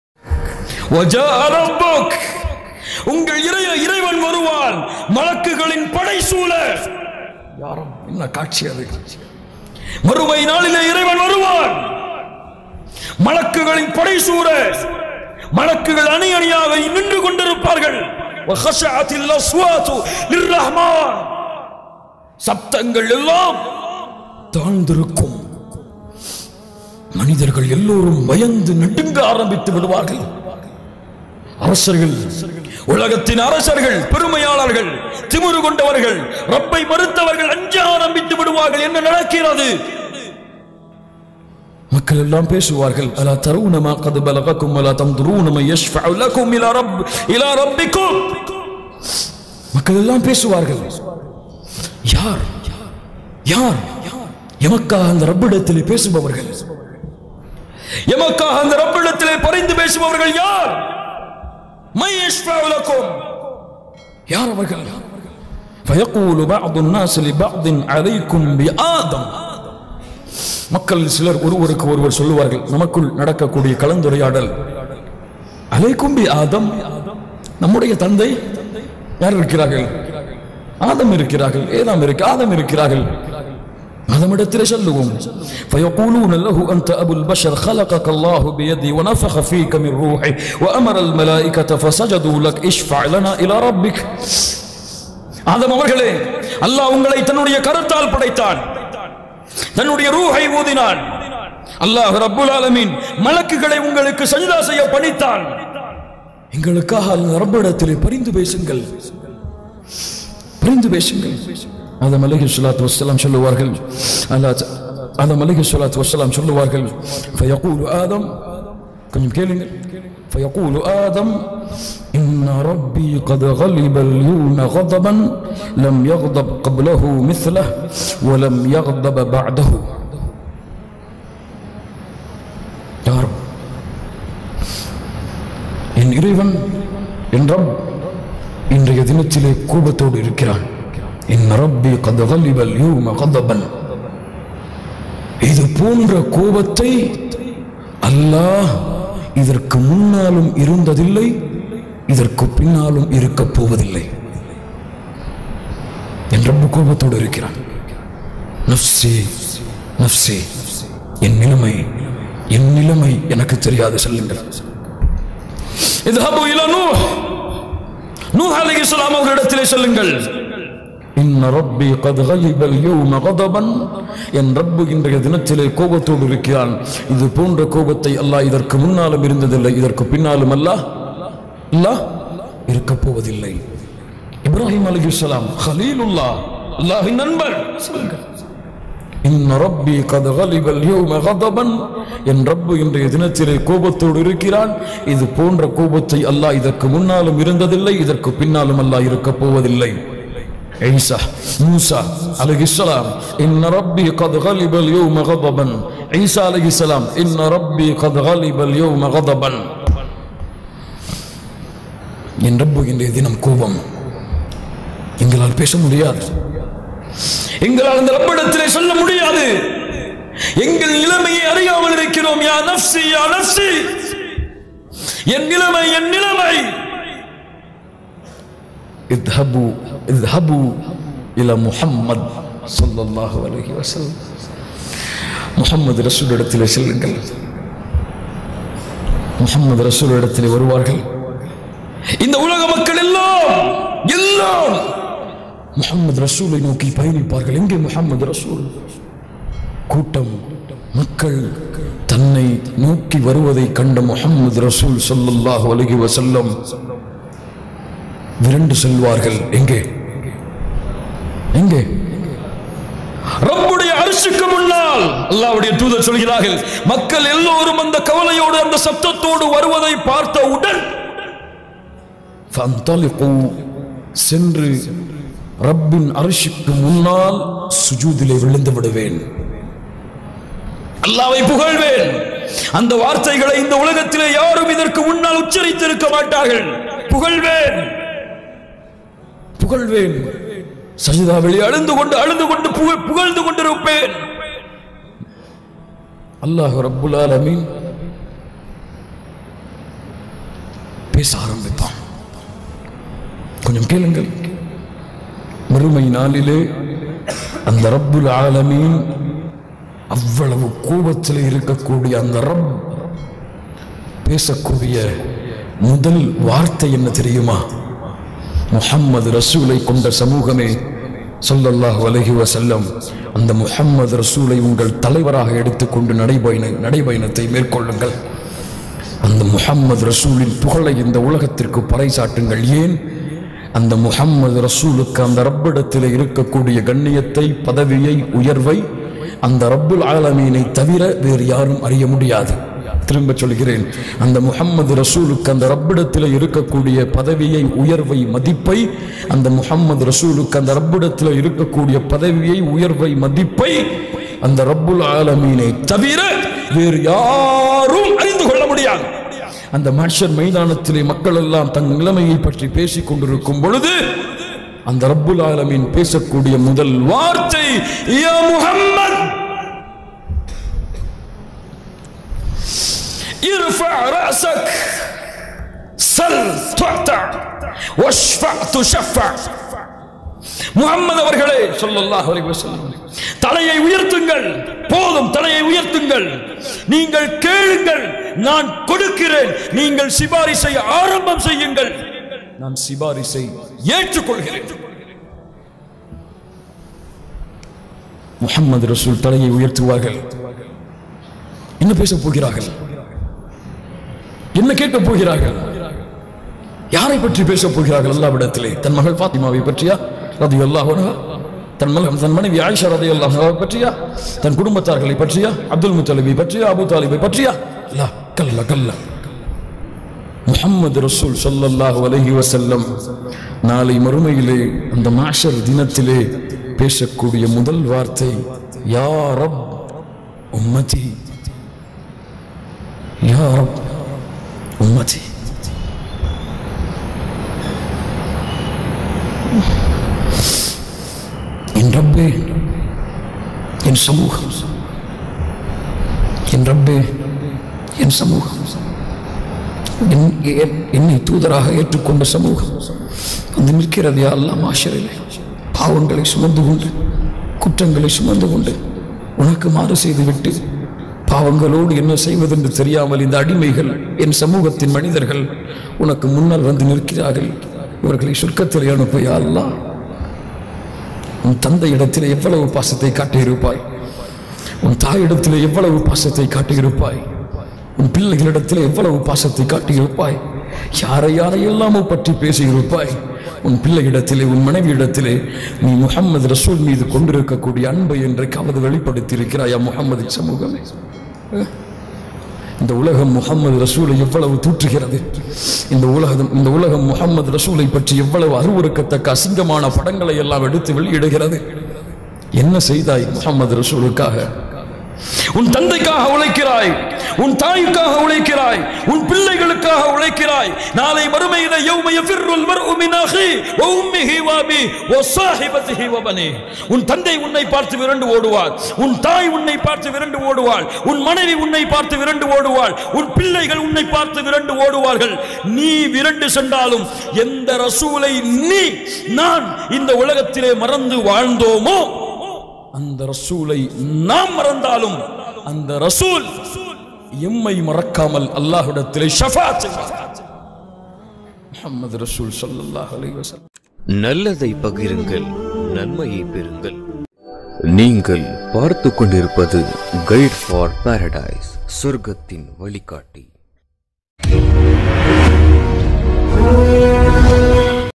மறுமையில் மனிதன் இறைவனை கண்டதும் என்ன செய்வார்கள் | Abdul Basith Bukhari | Guide For Paradise
இன்ஷா அல்லாஹ், ஒவ்வொரு நாளும் எமது YouTube தளத்தில் எதிர்பாருங்கள்.

நல்லதை பகிருங்கள்♻️ நன்மையை பெறுங்கள்😍

Jazakallahu khair❣